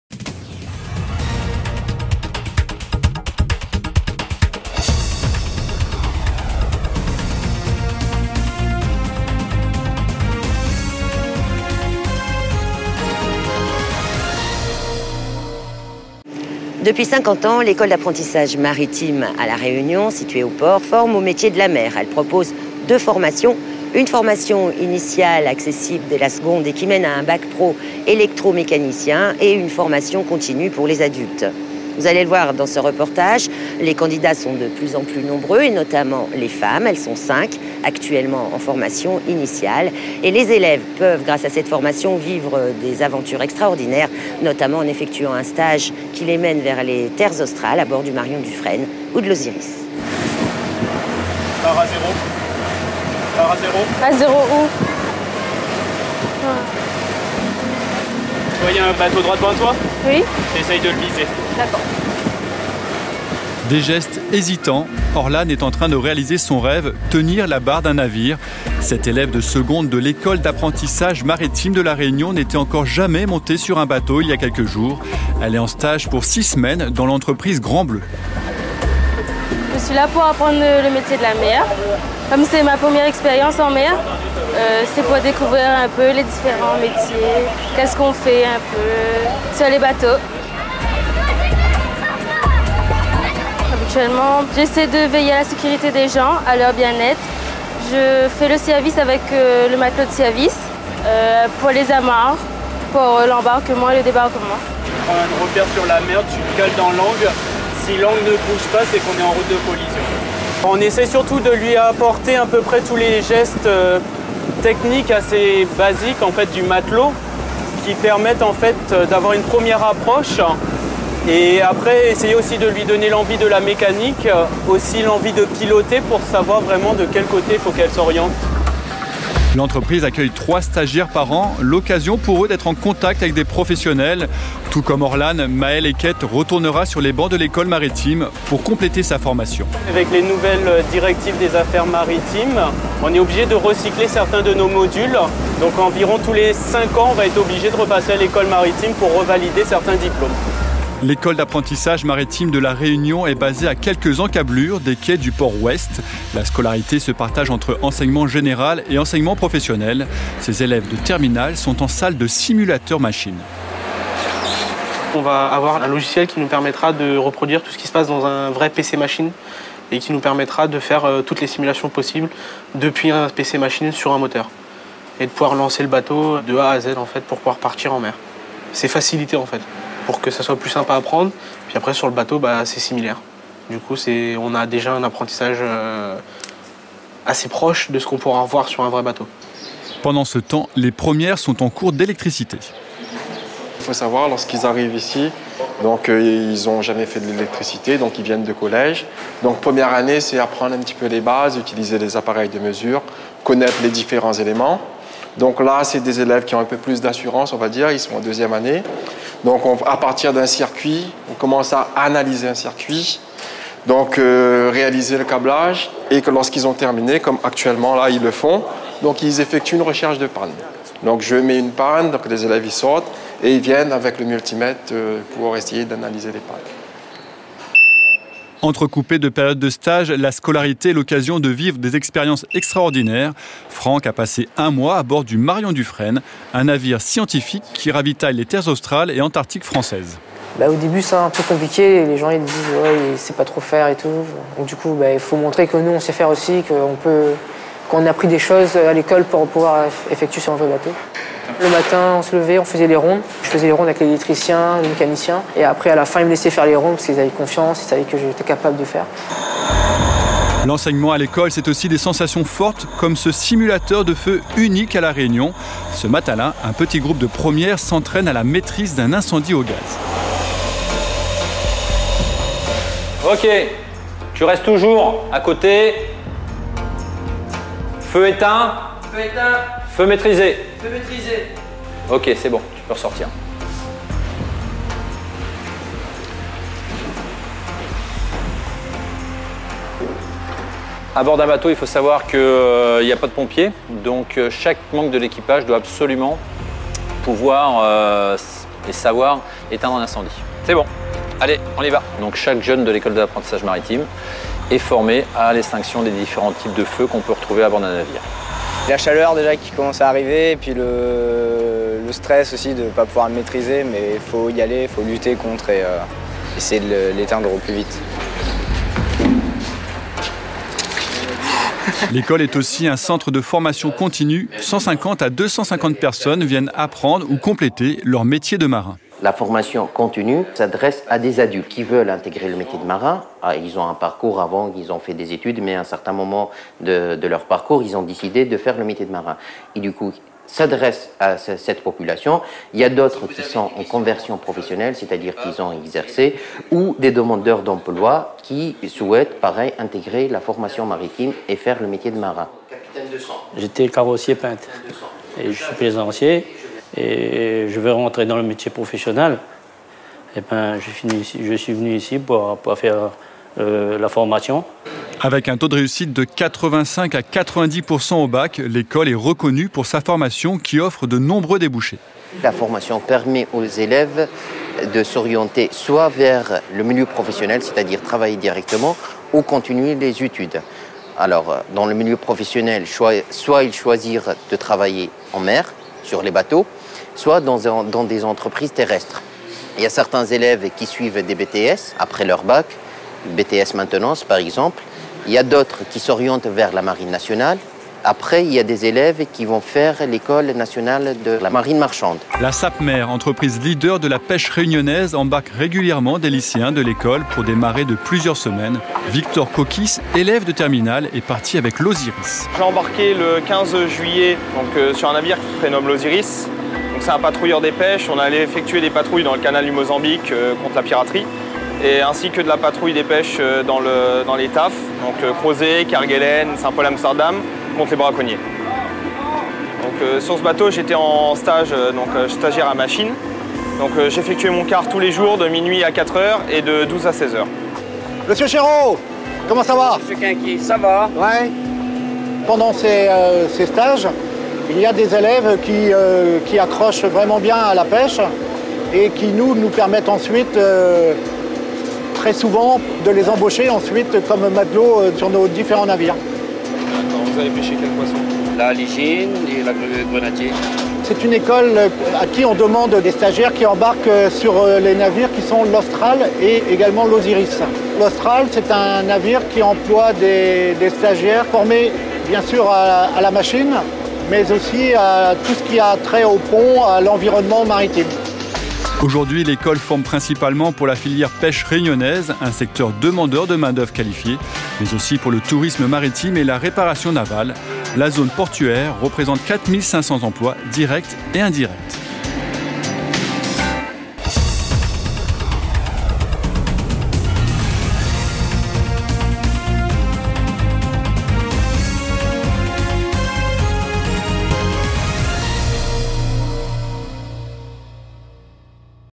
Ecole d'Apprentissage Maritime du Port (juin 2017)
Découvrez les formations proposées aux jeunes réunionnais par l'Ecole d'Apprentissage Maritime du Port.
(copyright ADIR, juin 2017)